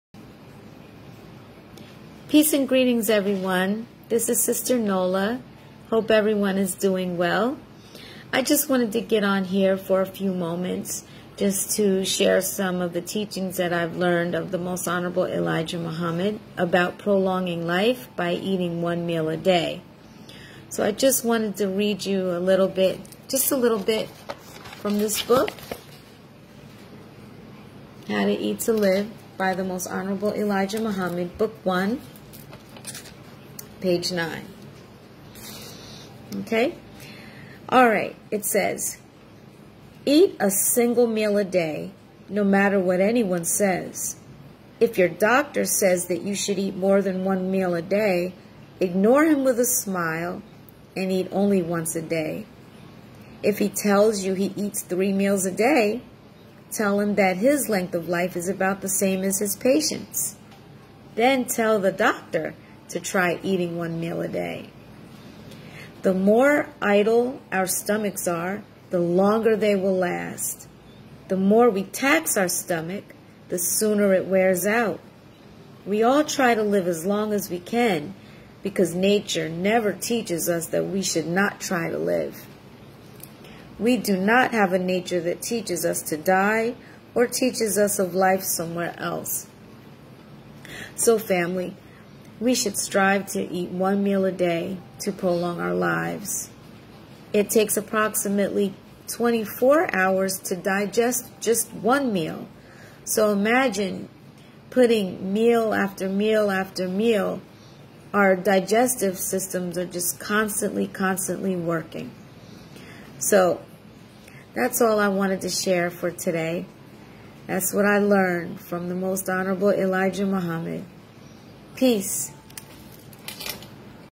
One Meal A Day Prolongs Life - How To Eat To Live - The Most Honorable Elijah Muhammad
The Most Honorable Elijah Muhammad teaches in His Book, "How to Eat to Live" that eating one meal a day gives our bodies enough time to digest the previous meal.  We overwork our digestive systems by keeping it constantly working to digest food. Health professionals are just now recently bearing witness to this Truth - they call it "intermittent fasting" .